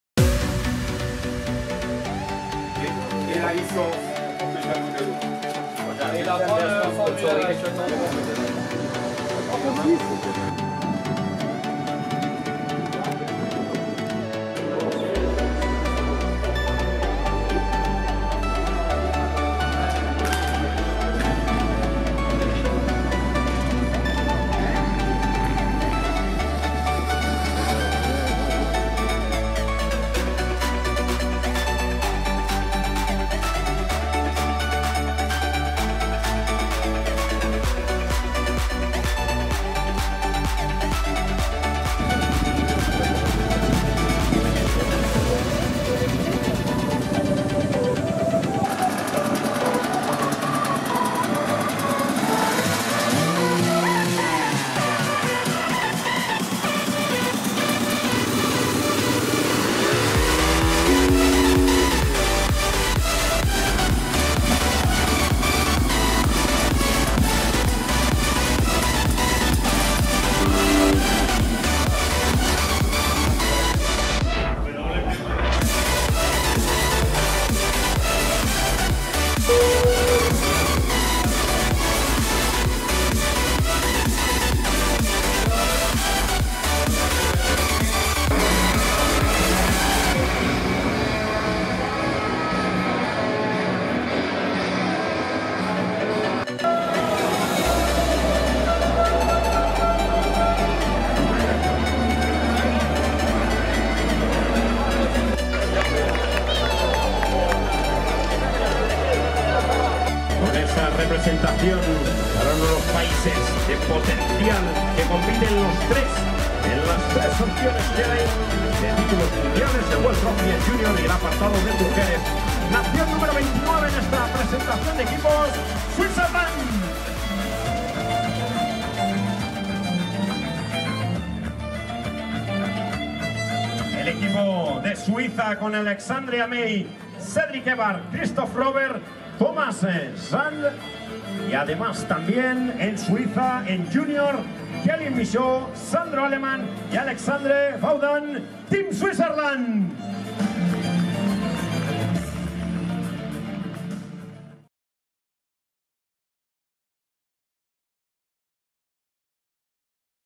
Swiss Team ISDE 2016 Contrôles & Cérémonie d'ouverture
Premières images de l'équipe Suisse aux Six Jours d'Enduro 2016 en Espagne, région de Navarre.

Cette année le team World Trophy est composé de 4 pilotes: Christophe Robert, Cédric Evard, Alexandre Amey et Thomas Scharantz.

Le Team Junior de 2 Pilotes: Alexandre Vaudan, Kélien Michaud et Sandro Allemann

Un seul Team Club avec 2 pilotes: Robert Kamber et Pascal Nardo.